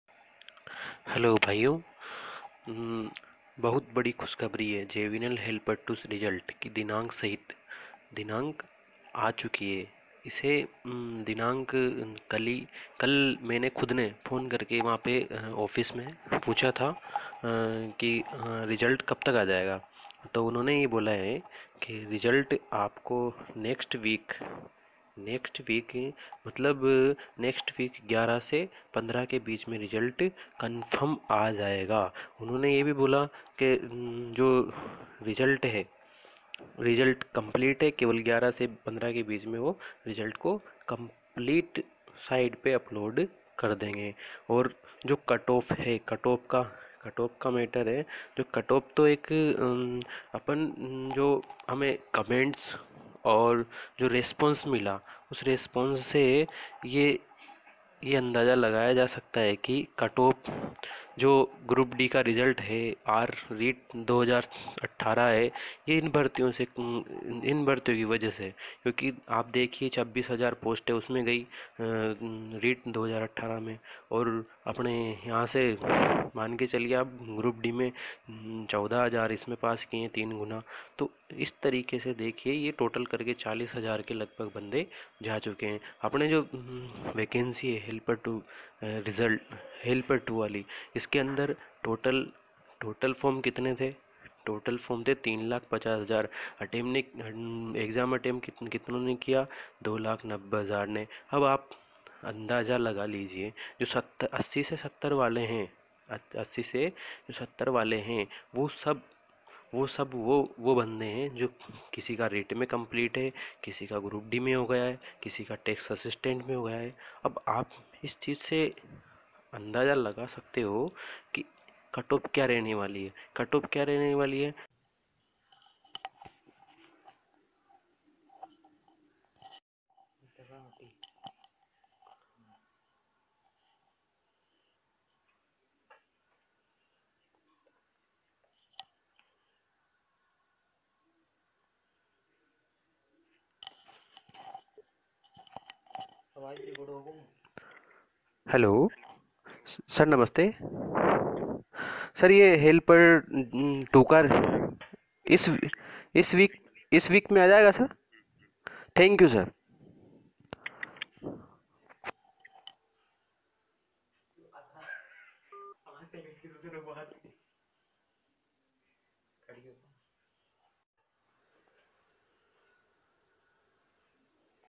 Jvvnl helper 2 results date announced | jvvnl हेल्पर 2 रिजल्ट डेट की घोषणा
Jvvnl helper 2 results date announced from jvvnl office